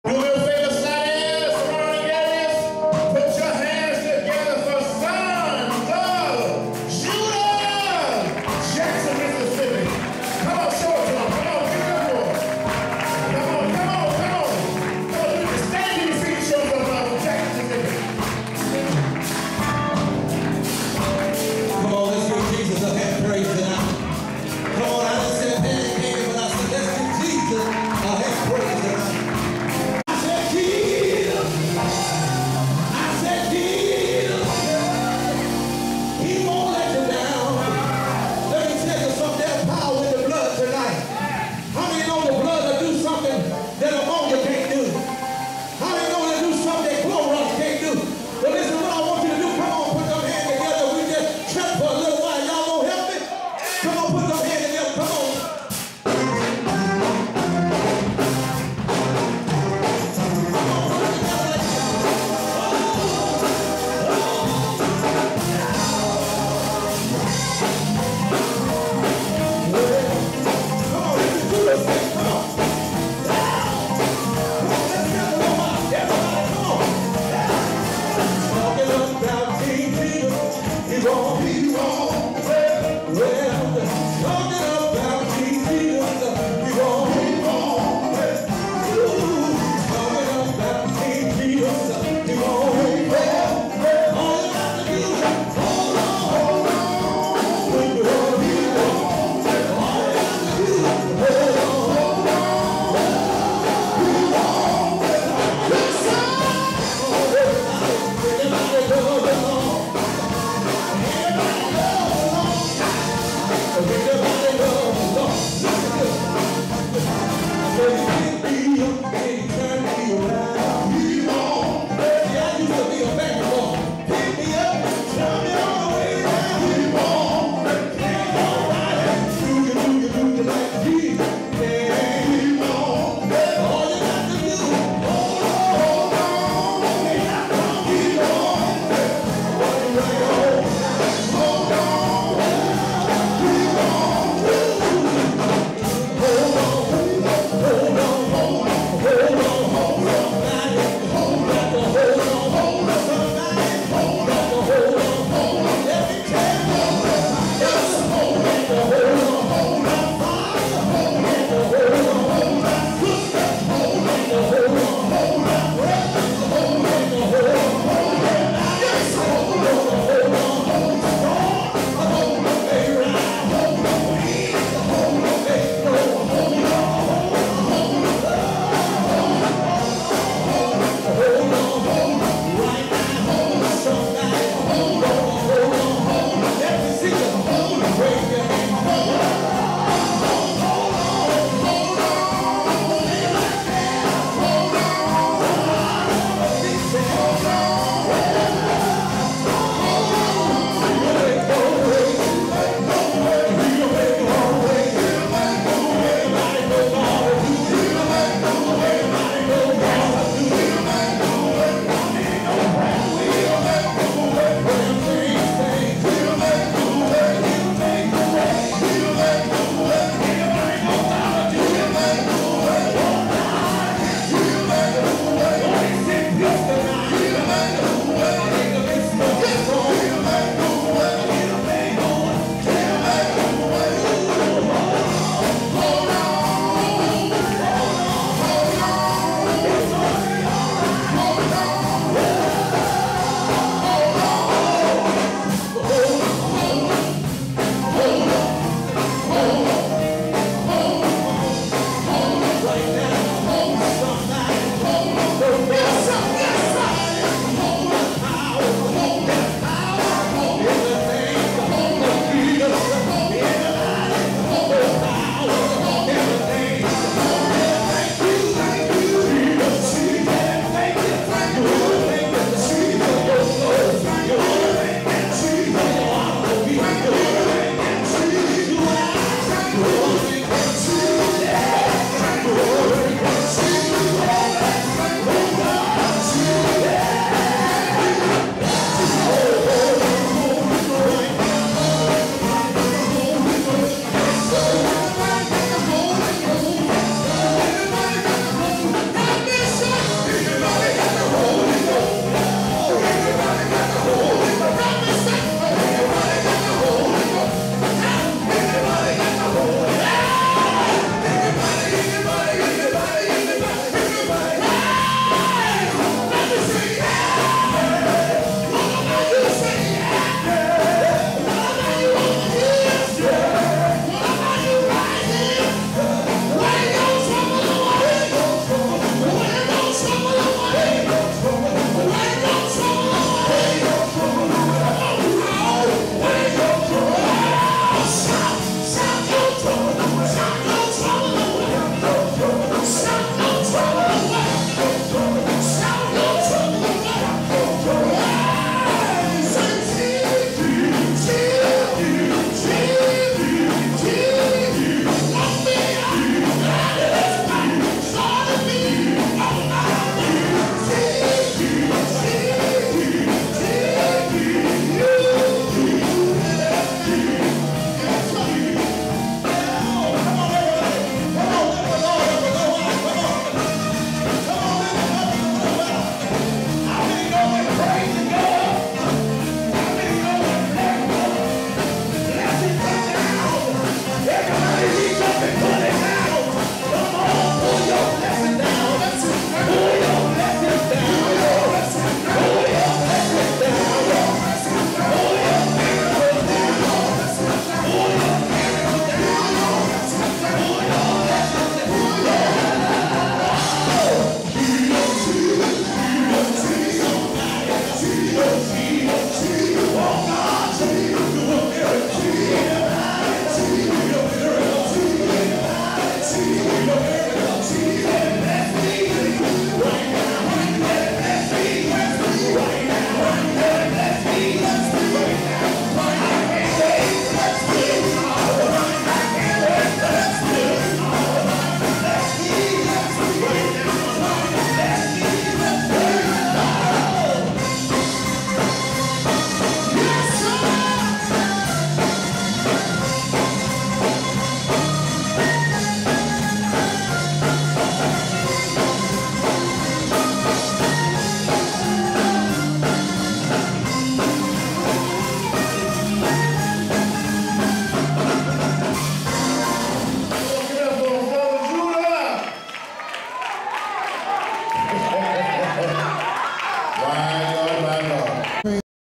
Sons of Judah in Slidell, La. for the Bester Singers Anniversary 2011
This is a clip of the Sons of Judah at the Bester Singers Anniversary in Slidell, La. on Sunday, April 17, 2011.